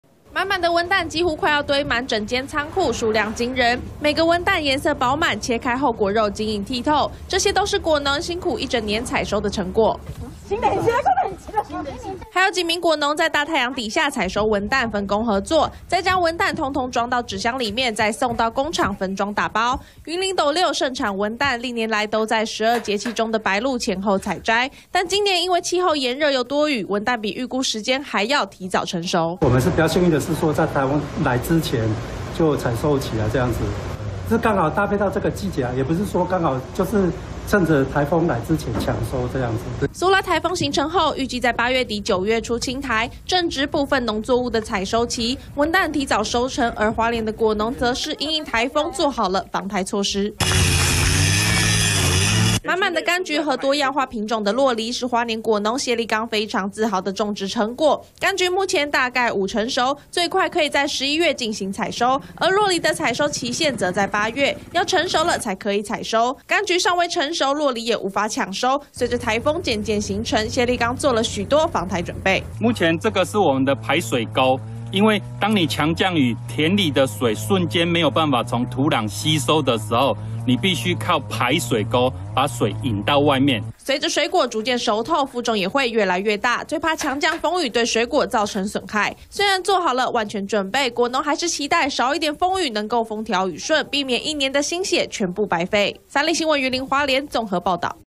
憂颱風月底侵台 果農護心血提早搶收文旦柚│94看新聞
雲林斗六盛產文旦，今年天氣炎熱加上水氣充足，文旦提早成熟，趕在颱風來襲前，果農今天搶先採收！但花蓮的柑橘和酪梨，都尚未成熟，一旦颱風帶來強降雨跟強風，一年的心血將付諸流水，農民也搶先做好防颱準備。

友邦連結區
◎ 訂閱【三立新聞網】最新影片搶先看